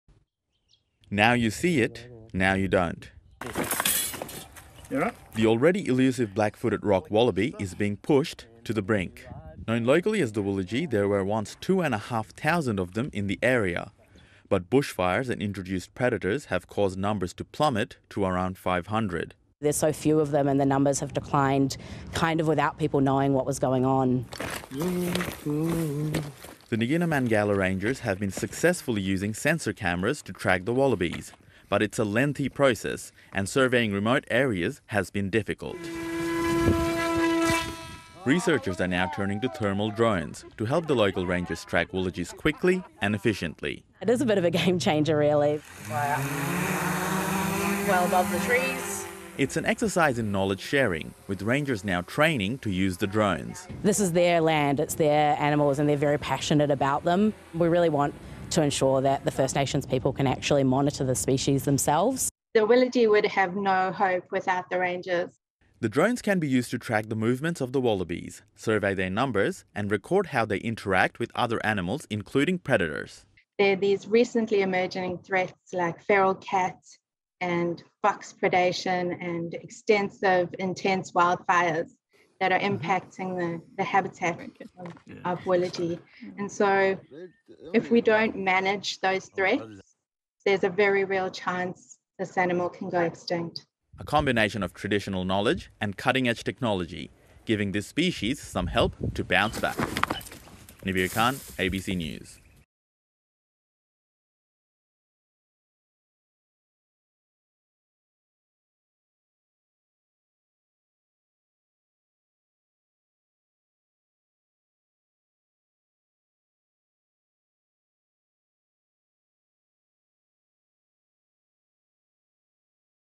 Drones used to track and protect endangered wallabies | ABC News
Thermal imaging drones are being trialled to track an elusive and endangered wallaby species in the Kimberley region of Western Australia.
Darwin researchers have teamed up with Aboriginal rangers in WA to see if the drones can provide a less costly way to track and protect the animals.